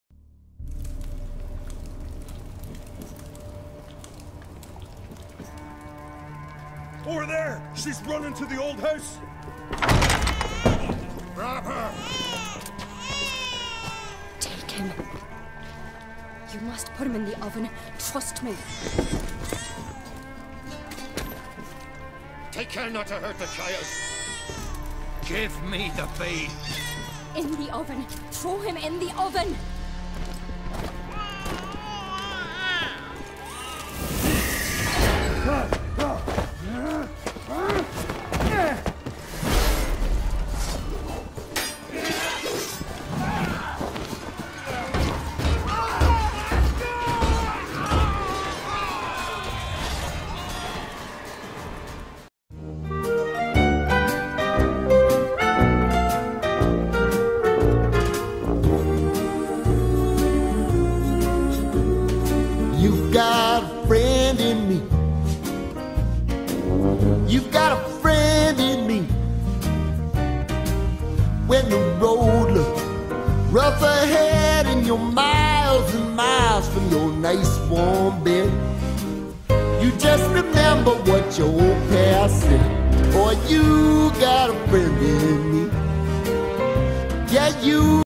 10/10 This Game is Fucking Fantastic
Witcher 3 shows that we STILL can have faith in games! Truly spectacular!